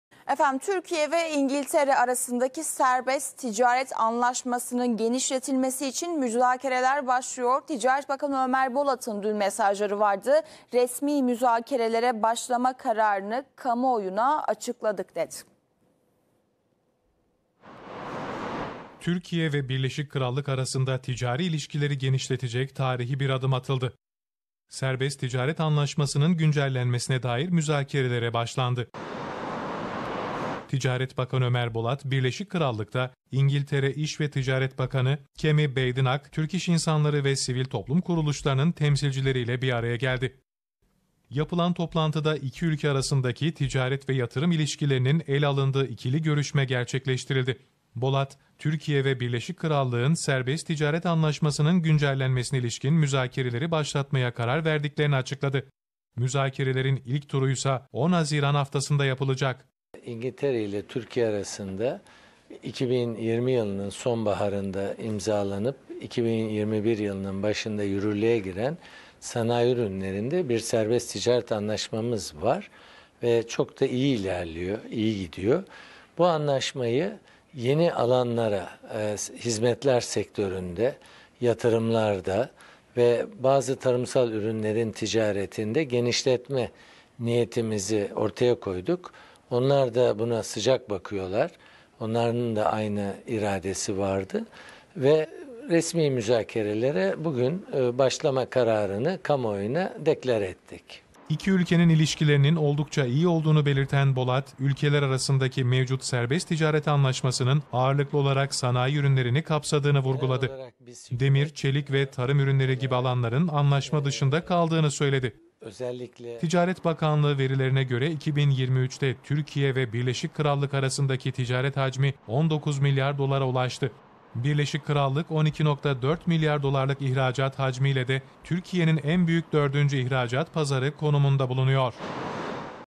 Bakan Ömer Bolat Duyurdu! "İngiltere İle Serbest Ticaret Anlaşması Güncellenecek" l A Para
Bakan Bolat: Serbest ticaret anlaşması güncelleme sürecini başlattık. Resmi müzakere başladığımızı bugün kamuoyuna deklare ettik.